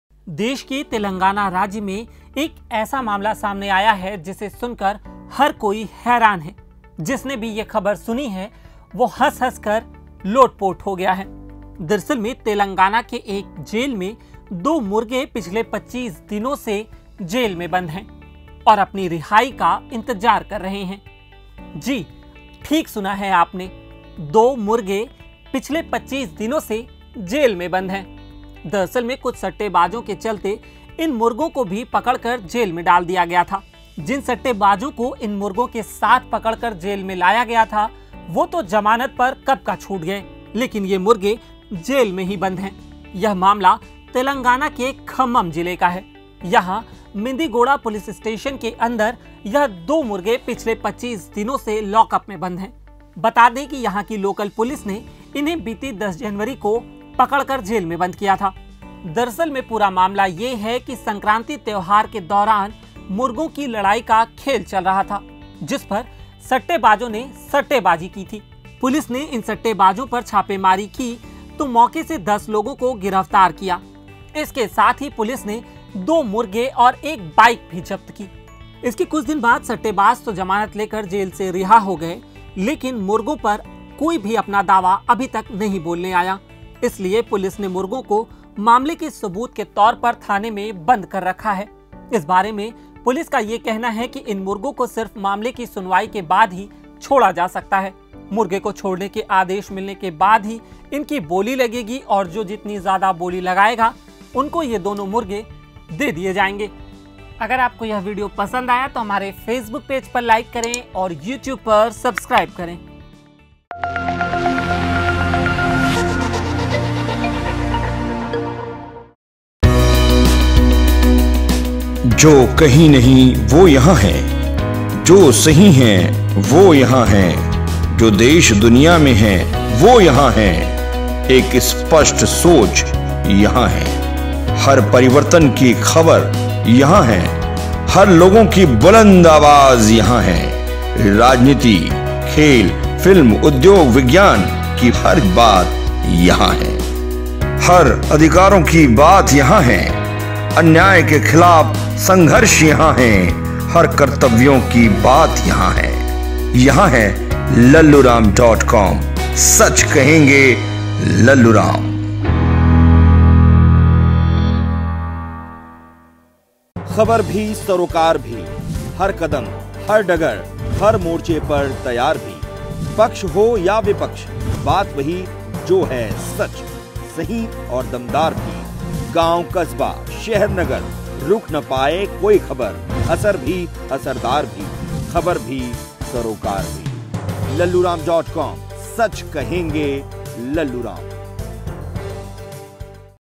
किस अपराध में जेल में बंद हैं यह मुर्गे? | Telangana Police | Cock Arrested
किस अपराध में जेल में बंद हैं यह मुर्गे?